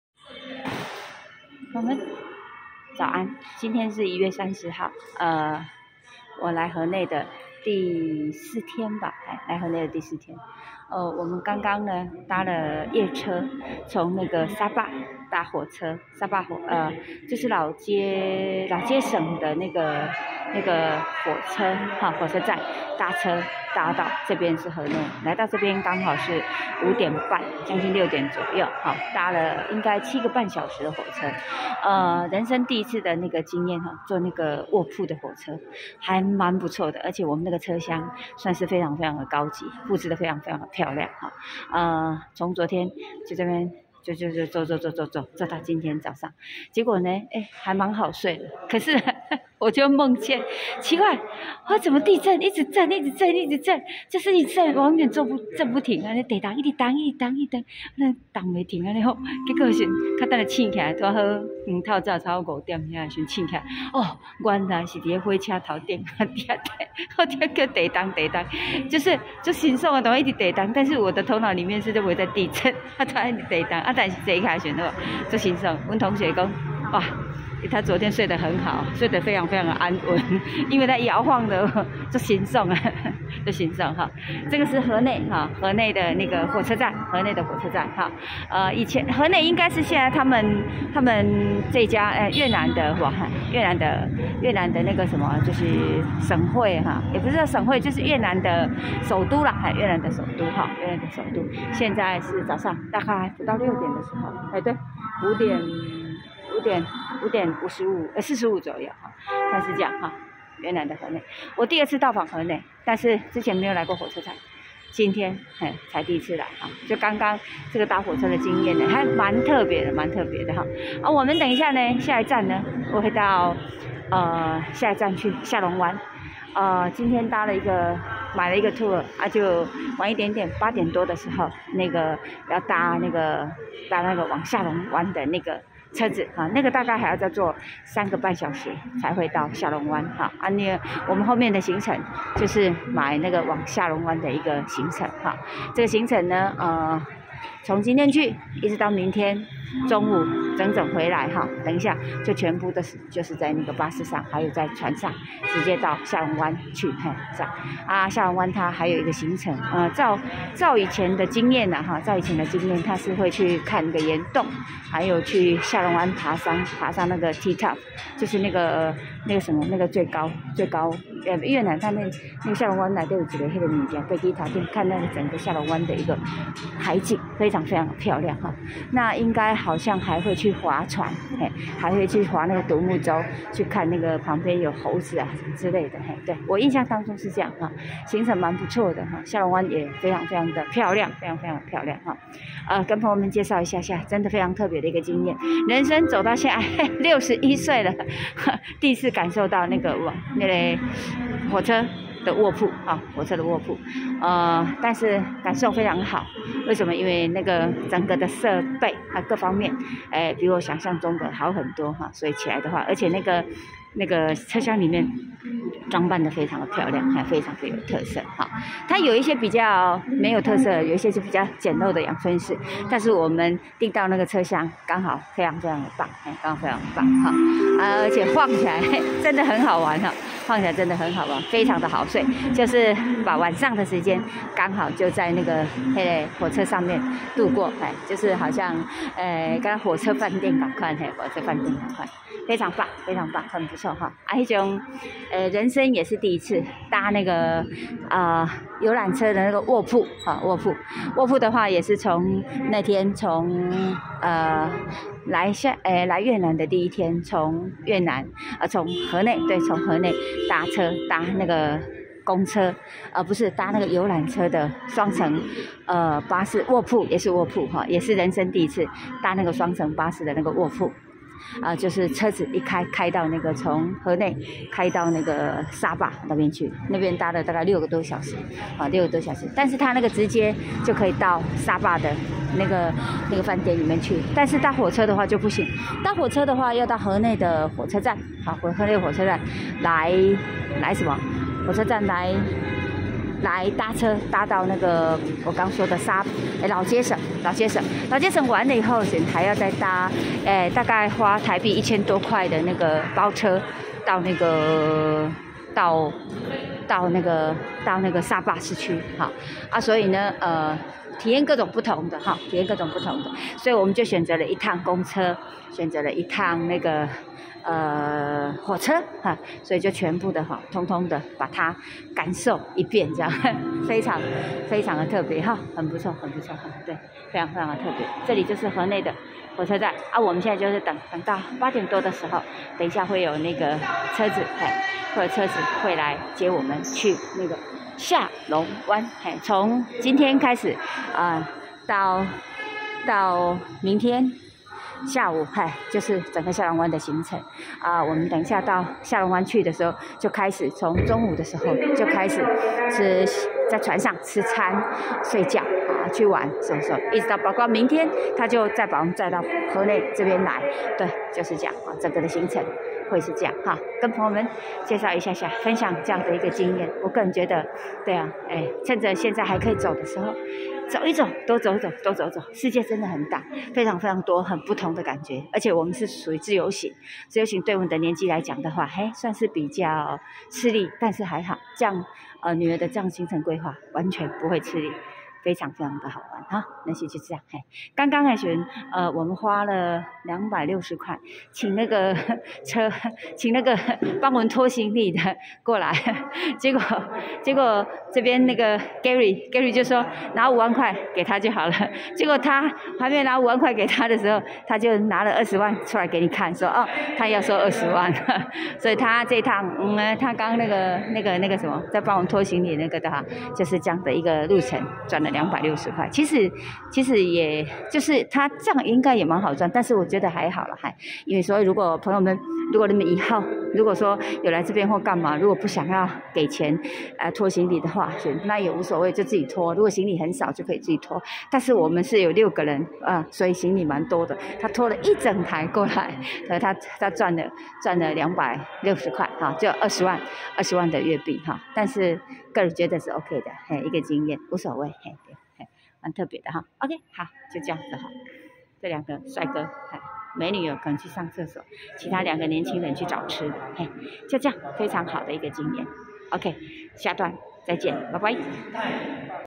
越南首都~河內火車站迎1/30第一道曙光！
112/1/30/陳美惠
行程：北越7日行
地點：河內火車站

坐了一夜的火車從山城沙壩回到了河內，剛好天快亮，接下來進行第四天的行程！

早餐後會有車子來接我們去搭往下龍灣的車，時間安排的非常的剛好，不但省下了一天的飯店費用，還體驗了生平第一次的臥鋪火車，真的回憶滿滿！